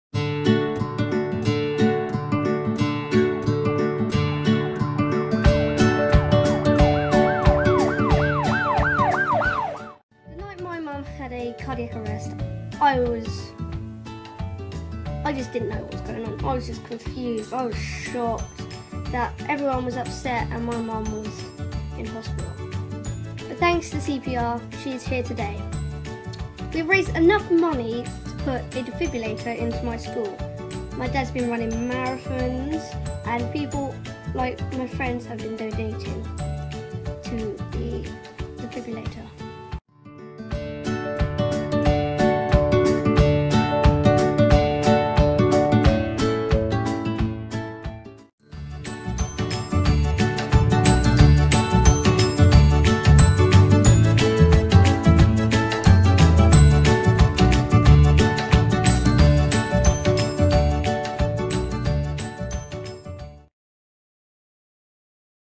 'Now is the Time' - Sudden Cardiac Arrest Experience
Tia, daughter of sudden cardiac arrest survivor Jean speaks on how she felt when she got the news of her Mum and how they're raising money for more accessible defibrillators